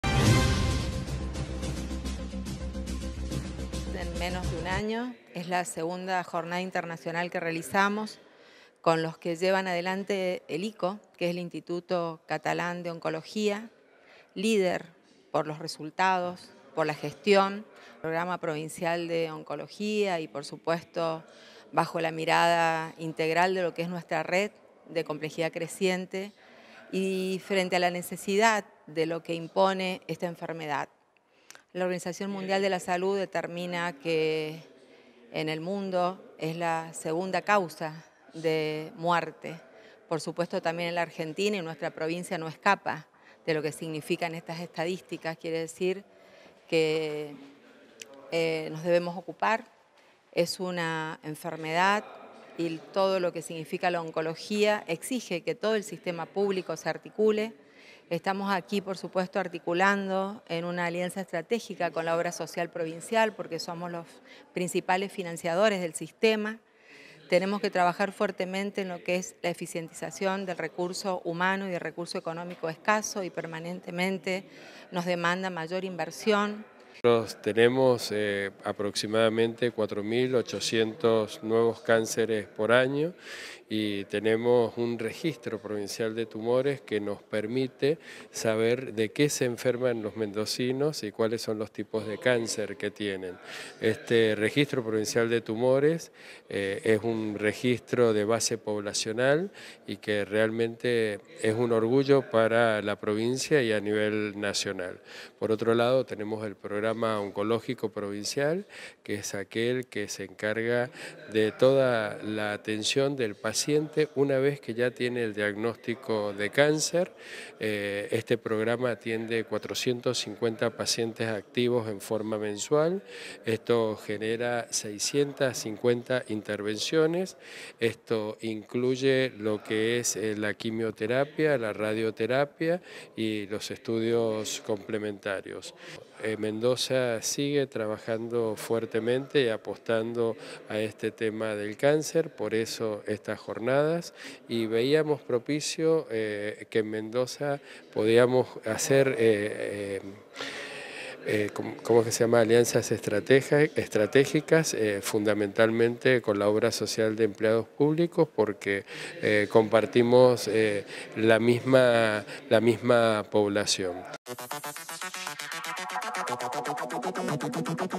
JORNADAS SALUDABLES
La Ministra de Salud Elisabeth Crescitelli participó de la 2° edición de las "Jornadas Internacionales de Oncología" que se llevó adelante en Mendoza. Participaron también directivos de OSEP.